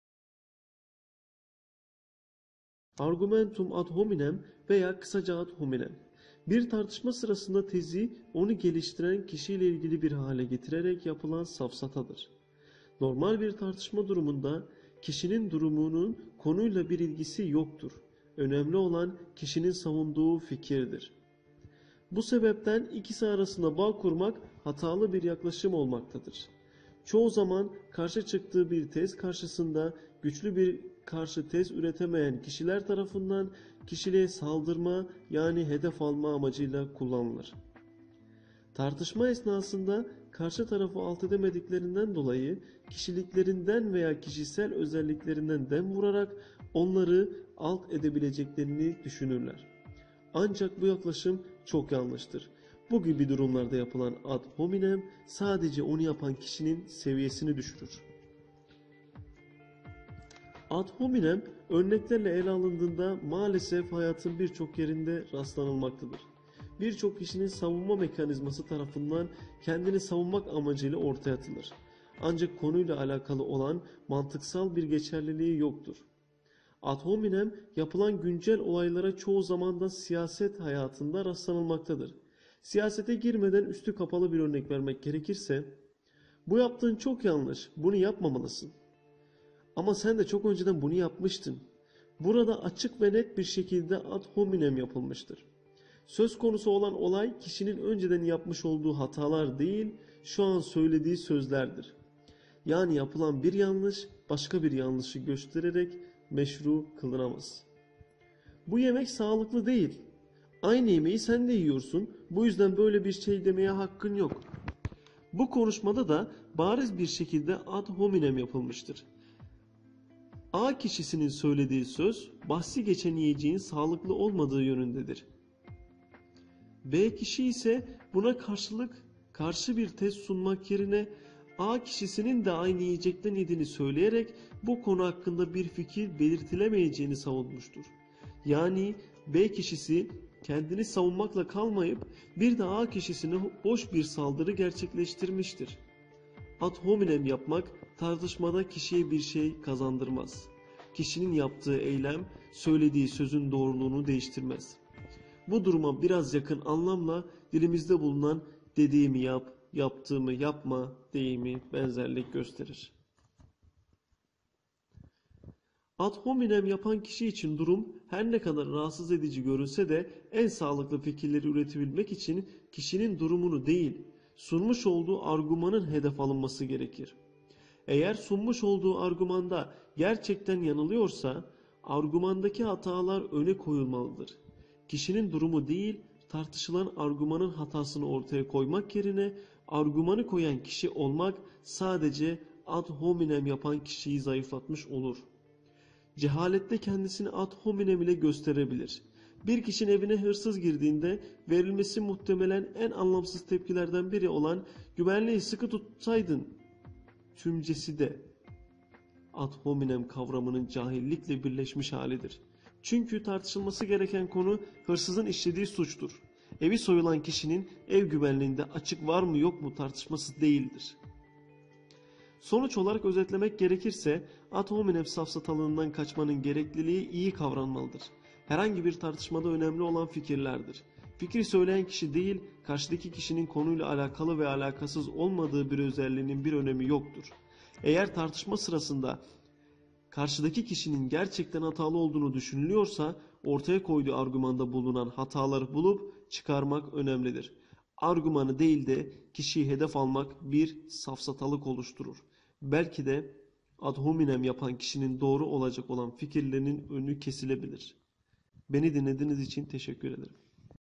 Günümüzün Yaygın Safsatası: Ad Hominem Nedir?
Argumentum ad hominem veya kısaca ad hominem; bir tartışma sırasında tezi, onu geliştiren kişiyle ilgili bir hale getirerek yapılan safsatadır.
Günümüzün Yaygın Safsatası: Ad Hominem Nedir? sizin için derledik.
Günümüzün Yaygın Safsatası: Ad Hominem Nedir? videomuzu sonuna kadar izleyin.

BU BİLGİLER SİTESİNDEN ALINIP VİDEO HALİNE GETİRİLEREK "DAVUT ALP" TARAFINDAN SESLENDİRİLMİŞTİR